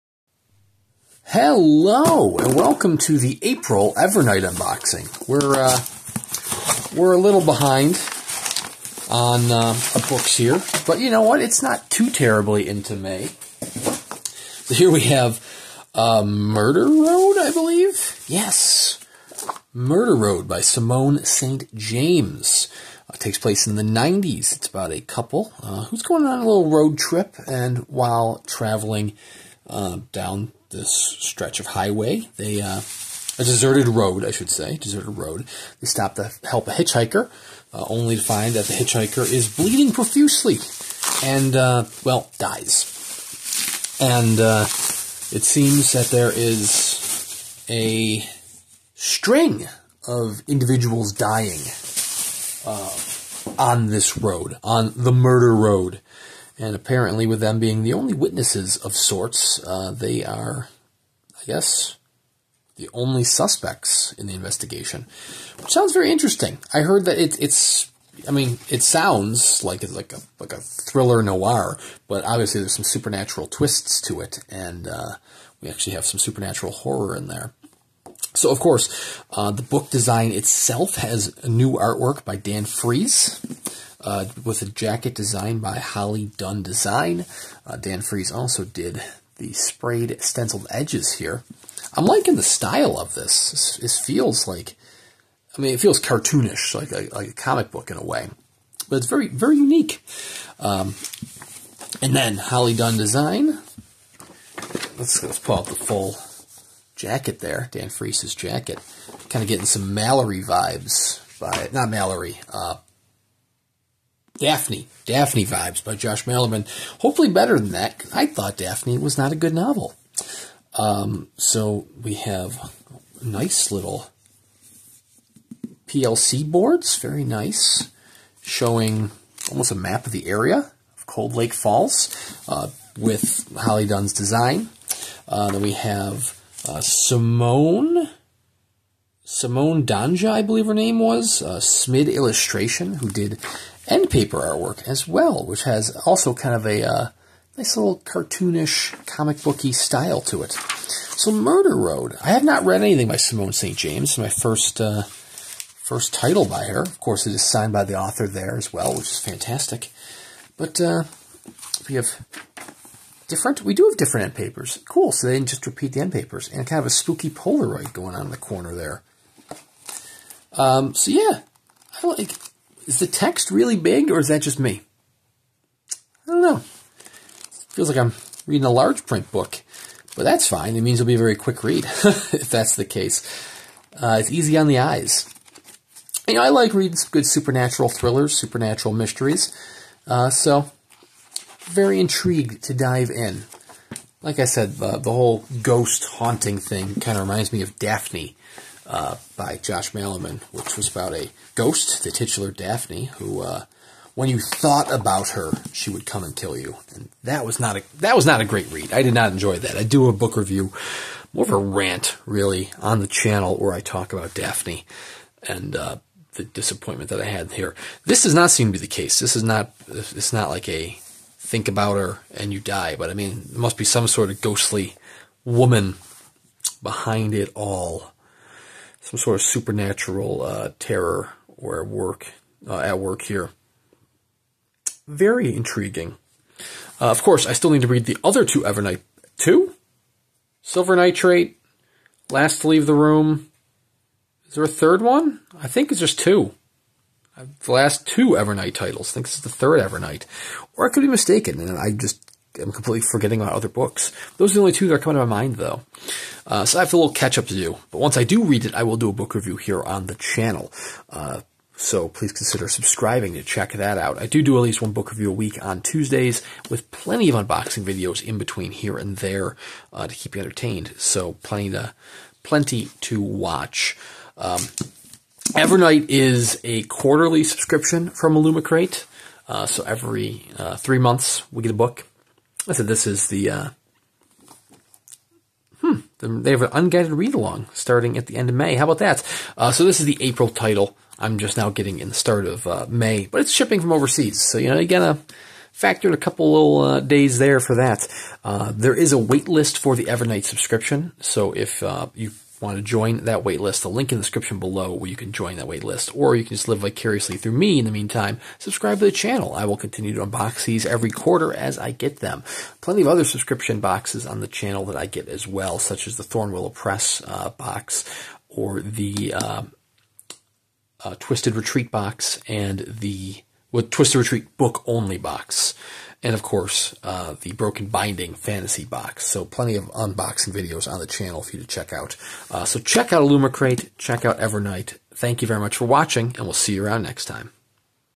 Unboxing Murder Road by Simone St James - Illumicrate Evernight Horror Box - April 2024
Unboxing Murder Road by Simone St James, the April 2024 book for Illumicrate's Evernight Horror Subscription Box.